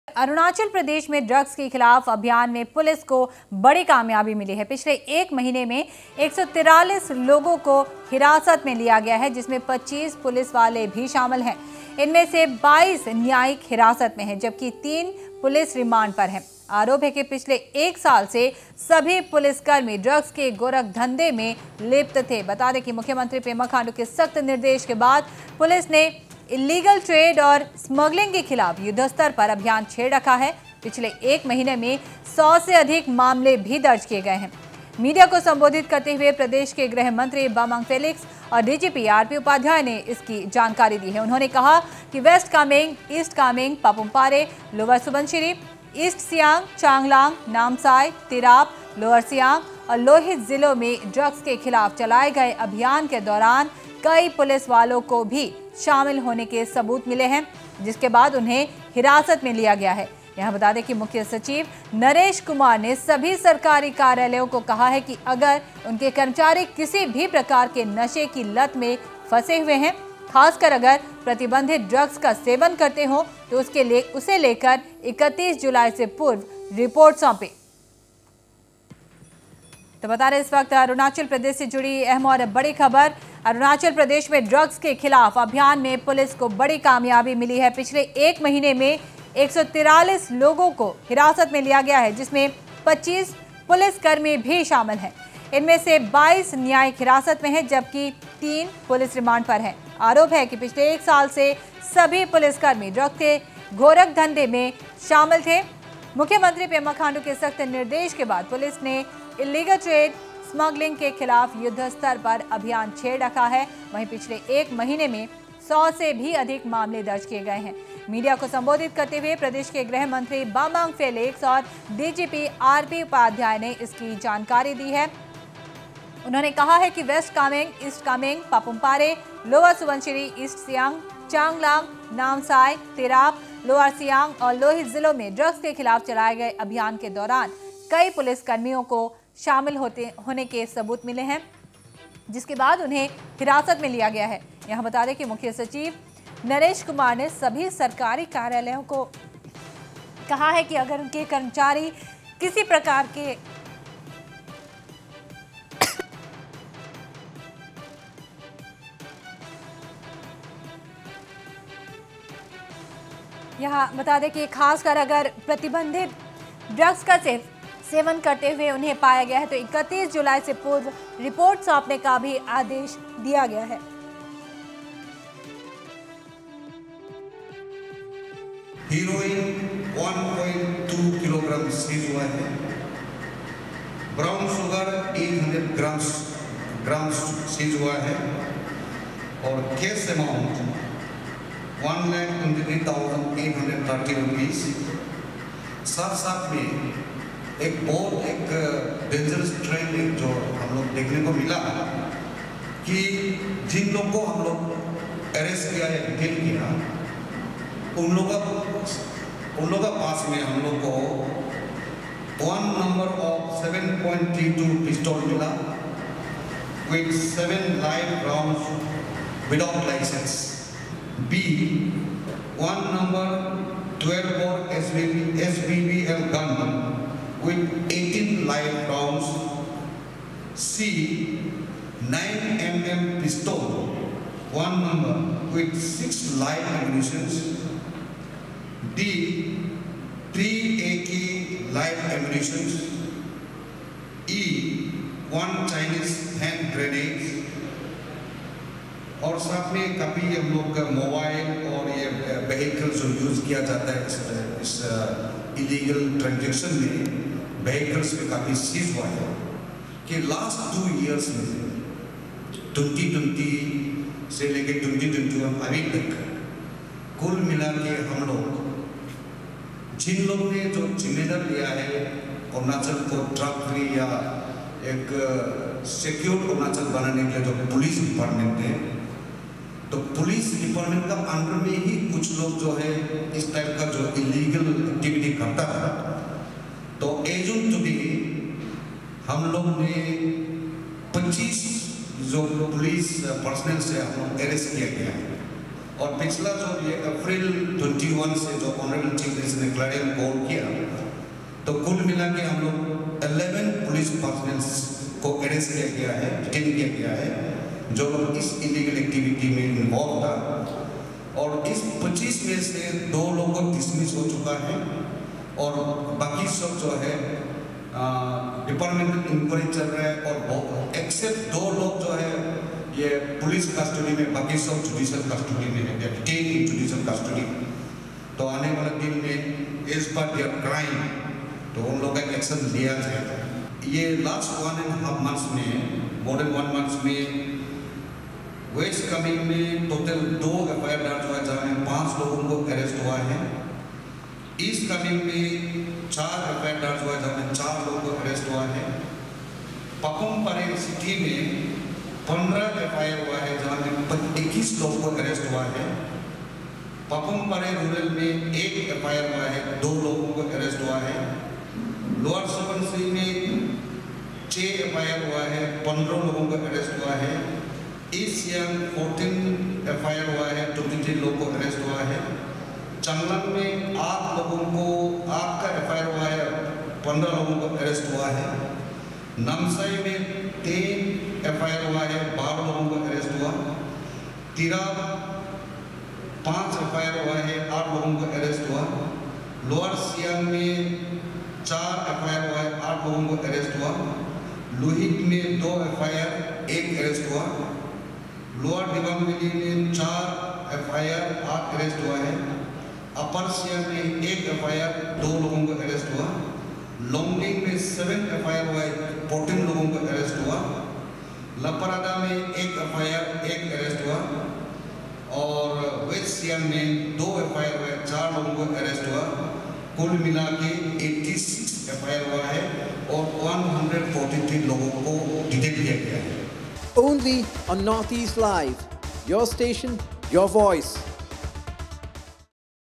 नशे के खिलाफ अभियान, अरुणाचल पुलिस को मिली बड़ी सफलता
अरुणाचल प्रदेश में ड्रग्स के खिलाफ अभियान में पुलिस को बड़ी कामयाबी मिली है। पिछले एक महीने में 143 लोगों को हिरातस में लिया गया है, जिसमें 25 पुलिस वाले भी शामिल है। इसमें से 22 न्यायिक हिरासत में है। जबकी 3 पुरिस रिमांड पर है। पिछले एक महीने में 100 से अधिक मामले भी दर्ज किए गए है।